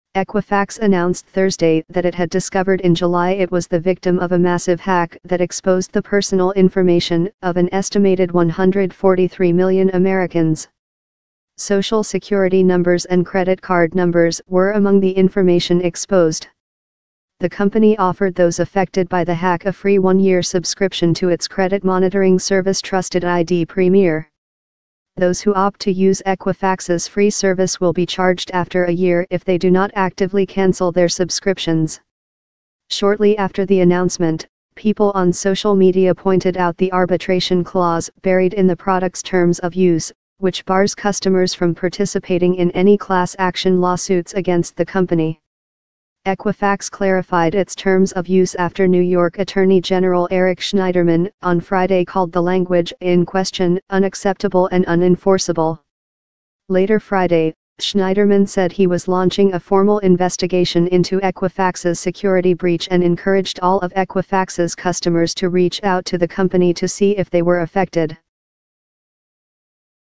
Equifax Clarifies Policy After Outcry Over Consumers Legal Rights Following Hack
Equifax announced Thursday that it had discovered in July it was the victim of a massive hack that exposed the personal information of an estimated 143 million Americans. Social Security numbers and credit card numbers were among the information exposed.